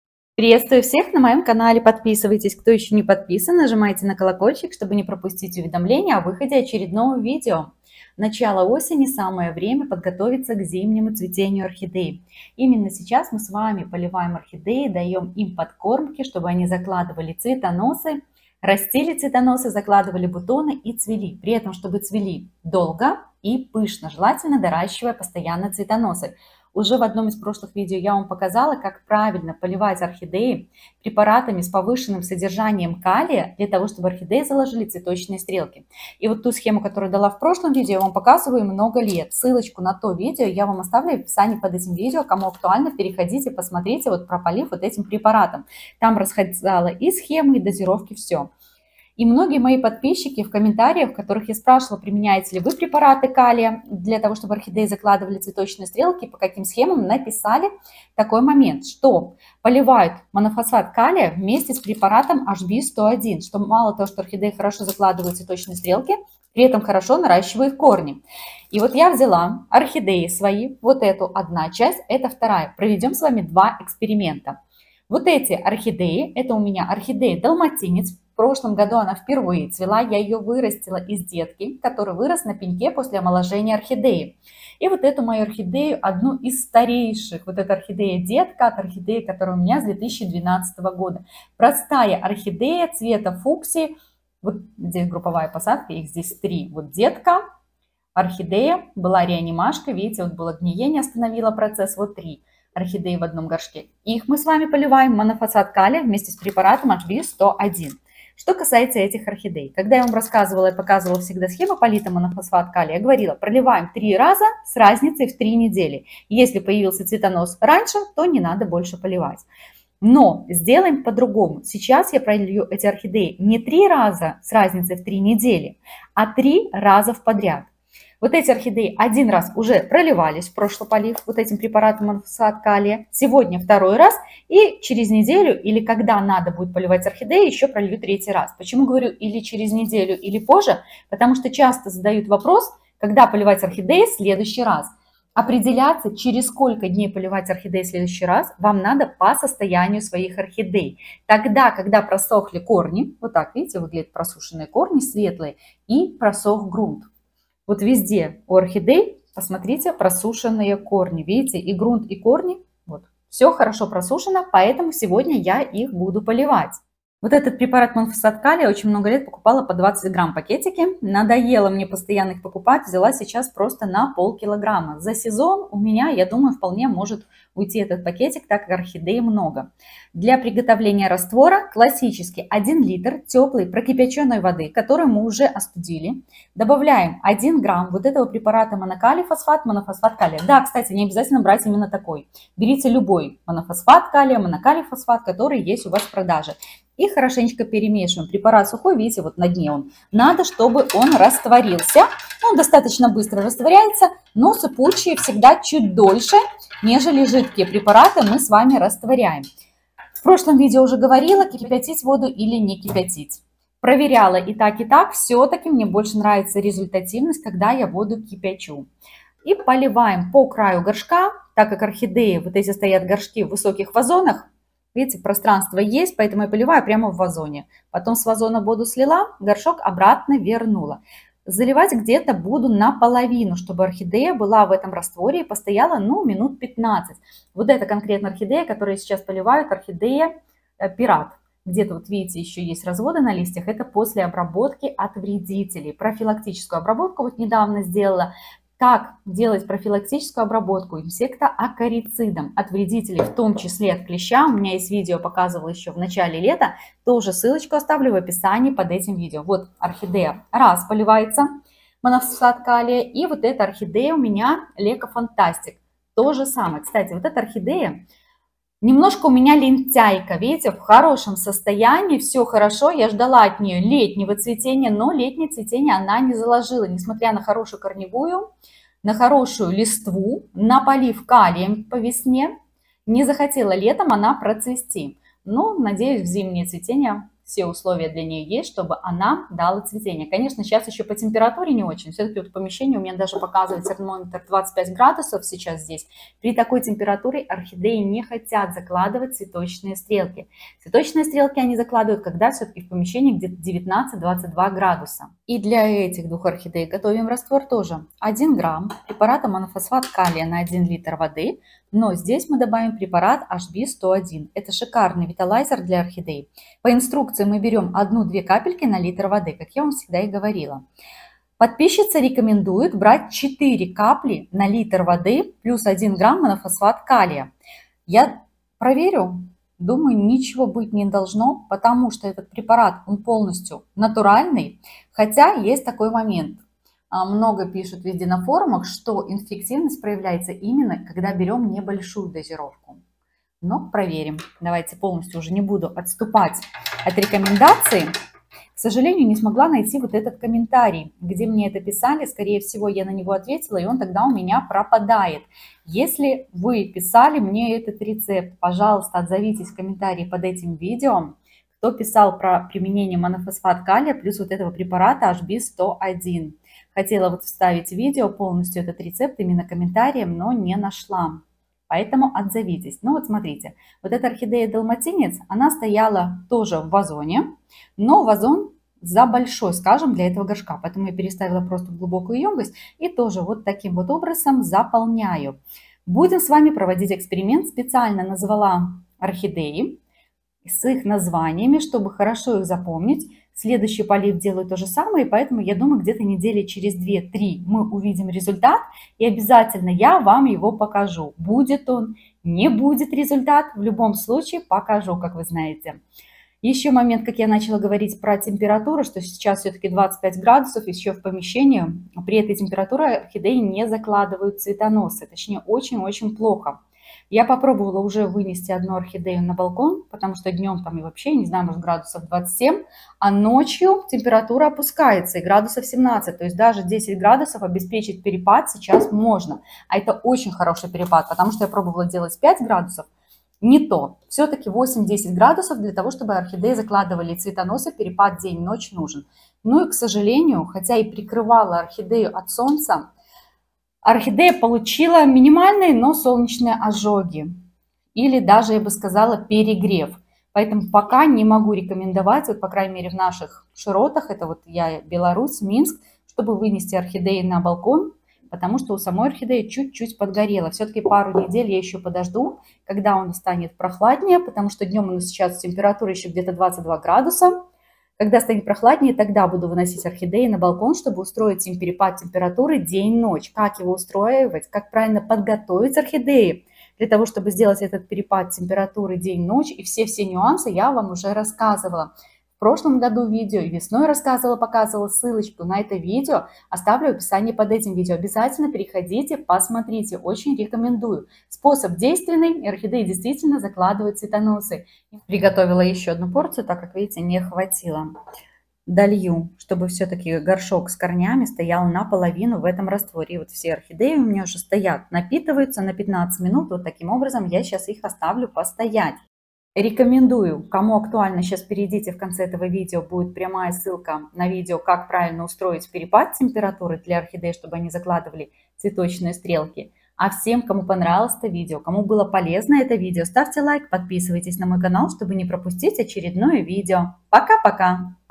Коктейли для цветения орхидей
Удобрения, чтобы орхидеи нарастили цветоносы и зацвели

Мой Instagram: orhidei_oh